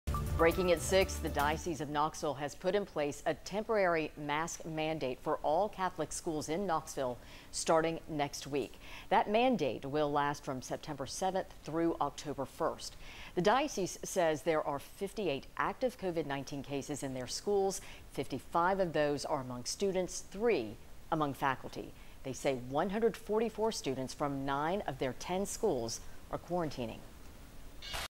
Mask mandate implemented in Knoxville Catholic Schools
The Diocese of Knoxville put a temporary mask mandate in place for all Catholic schools in Knoxville starting Sept. 7.